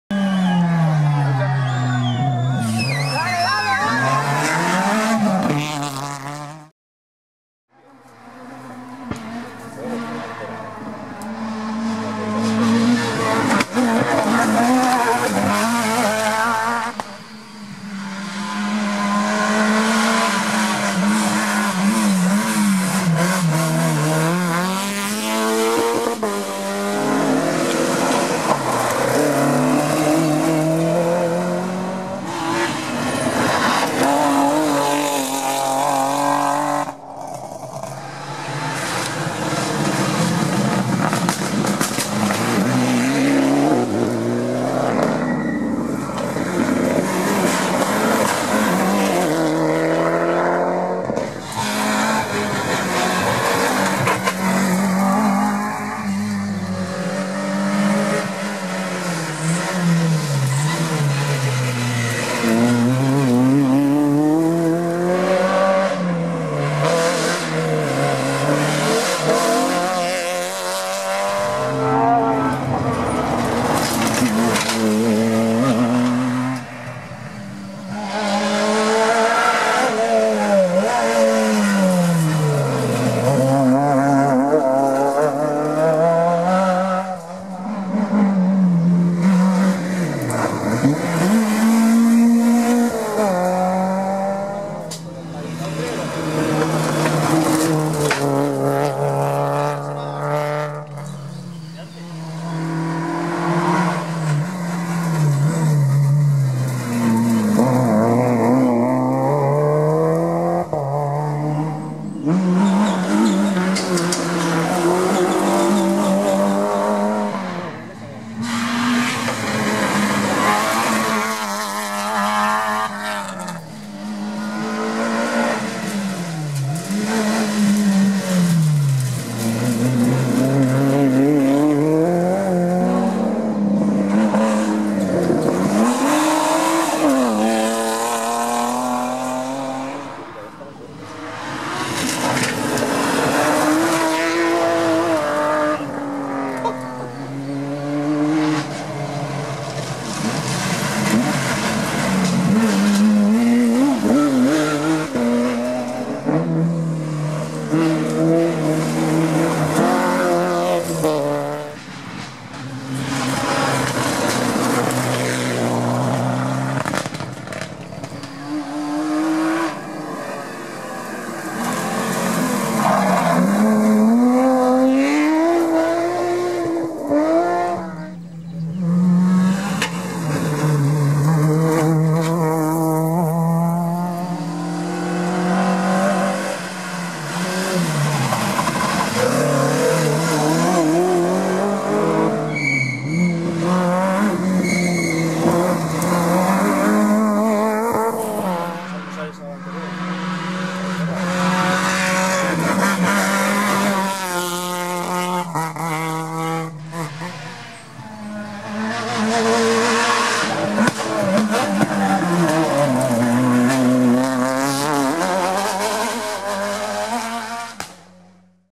Rallysprint Contrabandistas // Kontrabandisten Rallysprinta 2018
Aquí os dejamos una recopilación de los mejores momentos que hemos podido ver en el Rallysprint de los Contrabandistas.

Podium:

1º- Mattin Villares // Jon Zozaya  (Clio Maxi)

2º- Joseba Beola // Jose Luis Iratzoki  (Evo X)

3º- Alberto Marcos // Maider Usandizaga  (BMW M3)